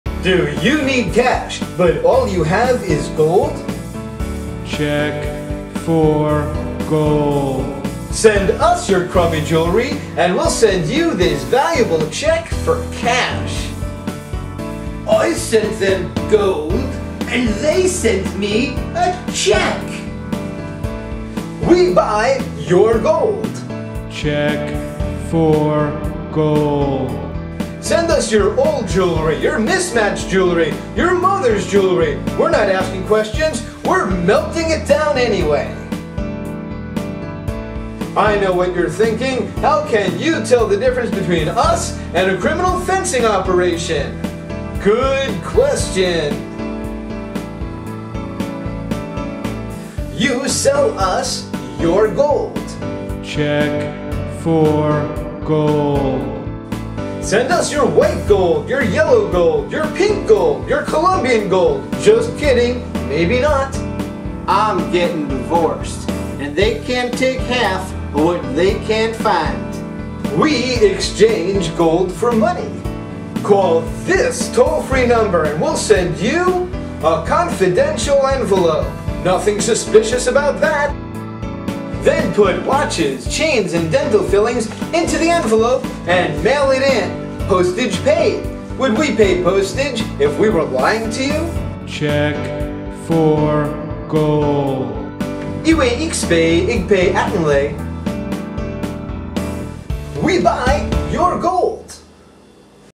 Check 4 Gold
Just a goofy sketch I shot at 4:00am one night. Got a chance to try out my fake moustache and the music generator on my video software.